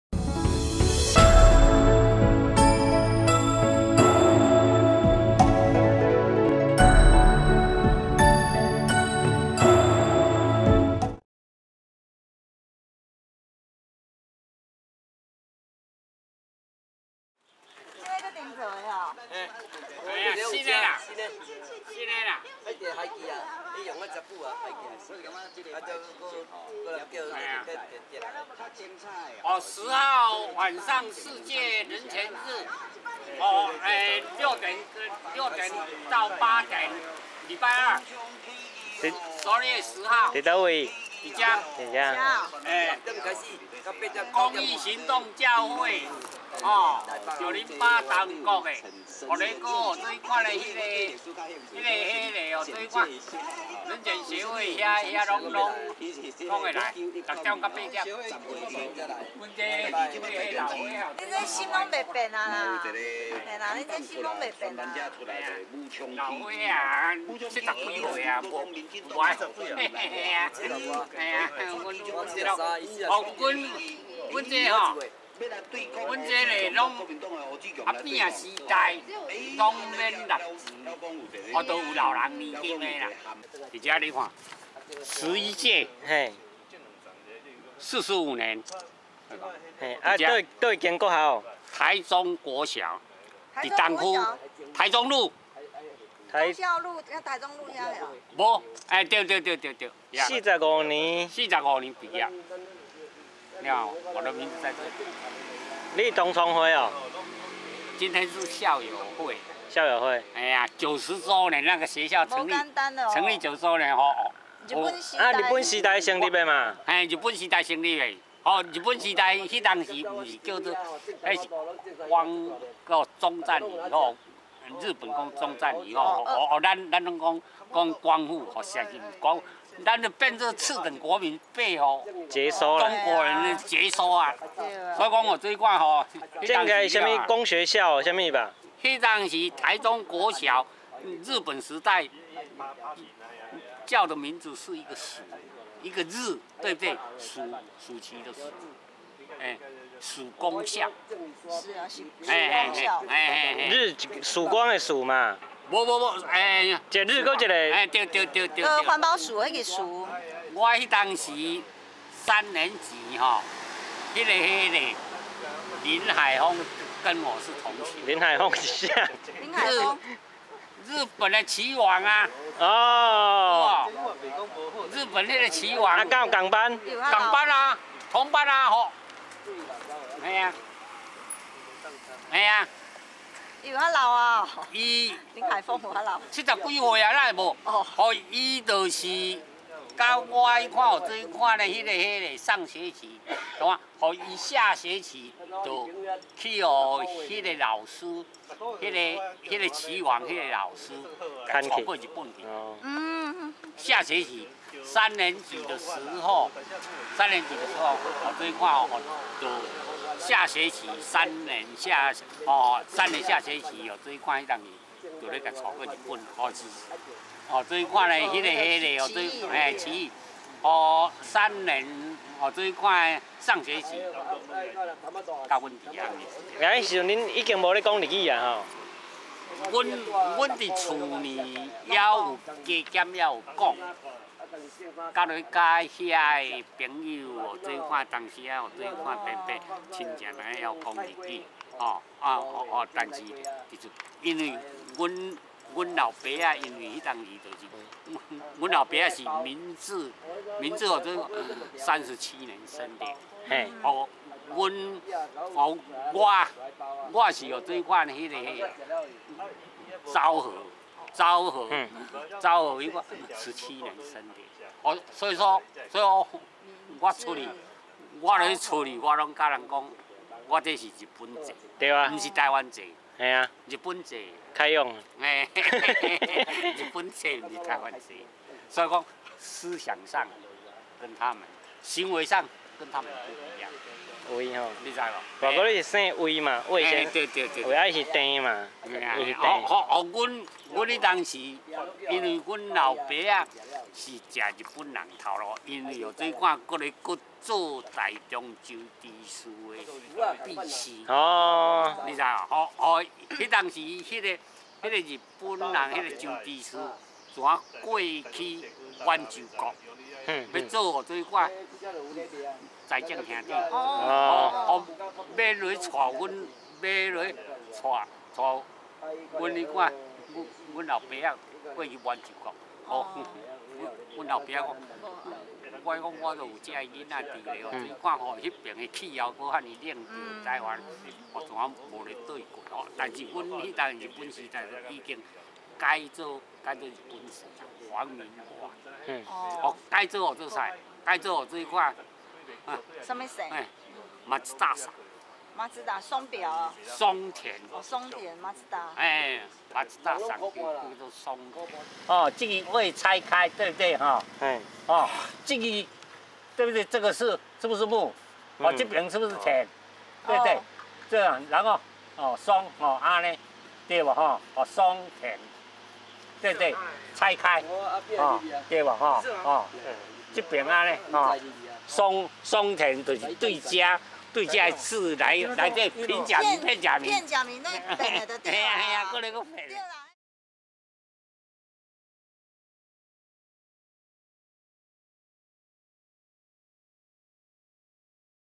台中監獄外－阿扁們的真情告白（7）20131207
2013年12月2日（週一）上午去幫陳總統送餐時，沒看到中監外的阿扁們鄭先生，到了下午才看到他，原來他感冒了，不過下午還是到中監外報到，今天2013年12月7日（週六）上午氣色不佳的鄭先生（73才）跟另一位魏先生（74才），兩人靜靜地坐在台中監獄外聽著廣播，關心台灣的前途，並呼籲12月10日（週二）世界人權日晚上（六點到八點）在台中監獄外，有聲援阿扁總統的晚會，希望大家來參加。

昨天facebook許多人轉寄一句話「抵抗未必是揭桿而起，堅持不懈，每個人都可以用自己適合的方式參與。」，鄭先生在他的摩托車後方貼了一面台灣國的國旗，魏先生則跟志工說前幾天中國海協會會長陳德銘來台中，他有去舉布條抗爭，被報紙拍到，兩位老人家正是堅持不懈用自己最熟悉的方法幫台灣爭取獨立與尊嚴。

閒聊之際，魏先生說他早上才去參加「台中國小」90週年的校友會，當年「台中國小」於日治時期名稱為「曙公學校」，魏先生當時還跟後來的世界知名棋王林海峰是同班同學。

魏先生還提到日治時期因為他父親任職於總督府的政府單位，所以在皇民化運動中家裡改姓為「松田」（Matsuda），「松田」就是「魏」字拆開又重新排列組合而選的姓。而他父親本來也要被找去「滿州國」作事，後來因為家有妻小，又覺得「滿州國」那邊太寒冷，所以後來沒去。

今天的採訪有如冬日裡的暖陽，溫馨中又帶有一點歷史感，其實，歷史不遙遠，可以從生活的土地、人物開始挖掘~